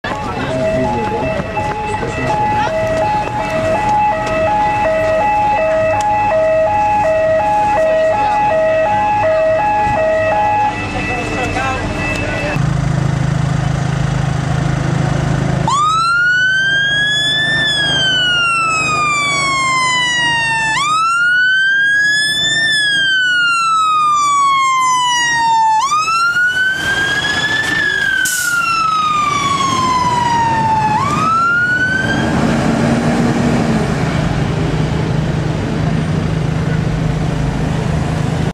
Evesham Ultra Heavy Rescue Pump Turnout - Hereford & Worcestershire Fire & Rescue Service
Evesham’s Ultra Heavy Rescue Pump Turning Out Of There Open Day With Blue Lights And Sirens To A “Make Pumps 6” Derelict Building Fire In Kidderminster Town Centre.

282 - VX12 GXM - Scania P280 Ultra Heavy Rescue Pump

©️Midlands999 2024